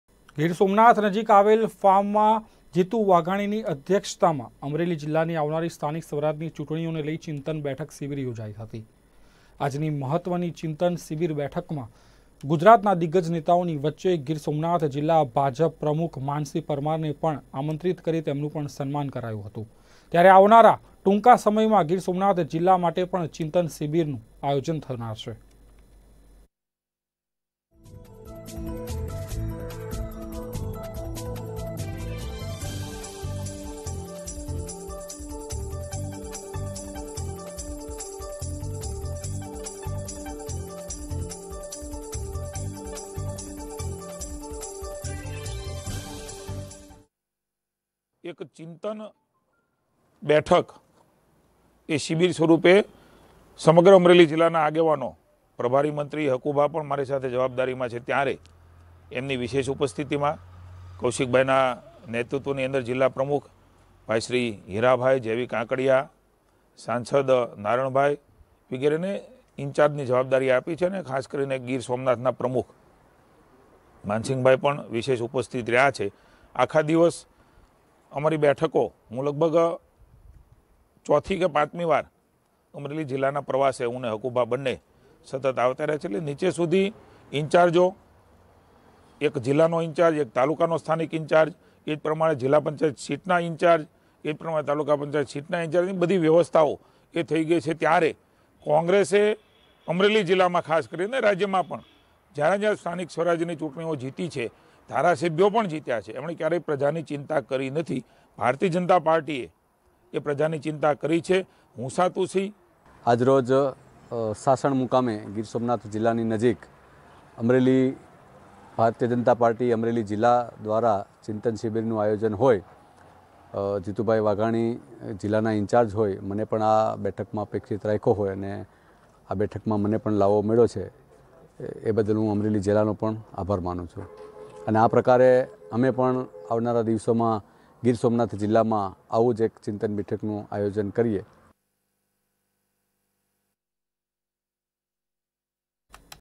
Girsomnath માં ભાજપની ચિંતન શિબિર યોજાઈ જિલ્લા ભાજપ પ્રમુખનું સન્માન કરાયું । Nirmananews
Girsomnath માં ભાજપની ચિંતન શિબિર યોજાઈ જિલ્લા ભાજપ પ્રમુખનું સન્માન કરાયું । Nirmananews watch live news on :